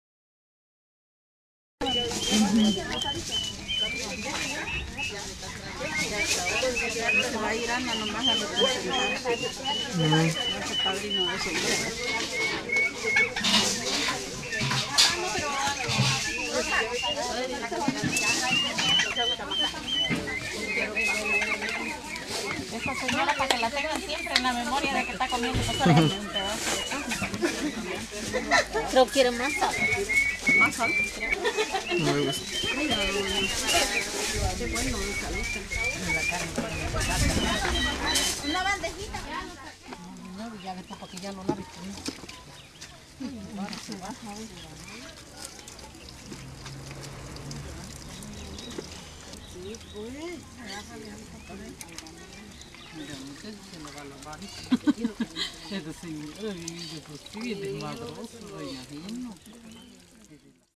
Cocineras de El Pericón
De las mejores cocineras que hay en el pueblo, preparando la barbacoa para una boda. [video del 2013]